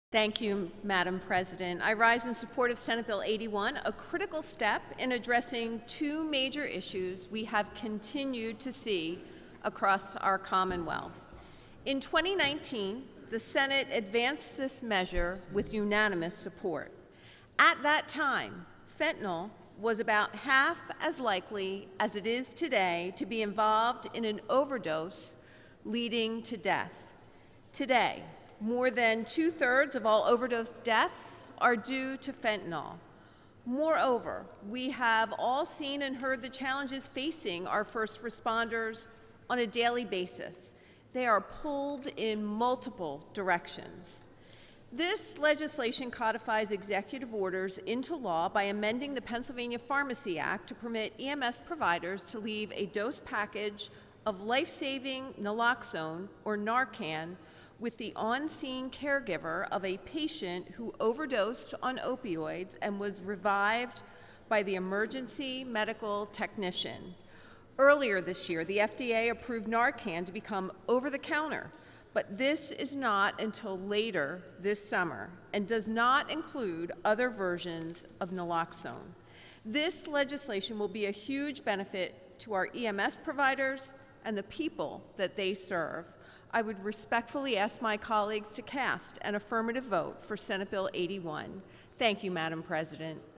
06.27.23 Senate Bill 81
Sen. Kristin Phillips-Hill (R-York) speaks in support of Senate Bill 81 during Senate session. This legislation would allow Emergency Medical Service (EMS) providers to leave life-saving drug with caregivers.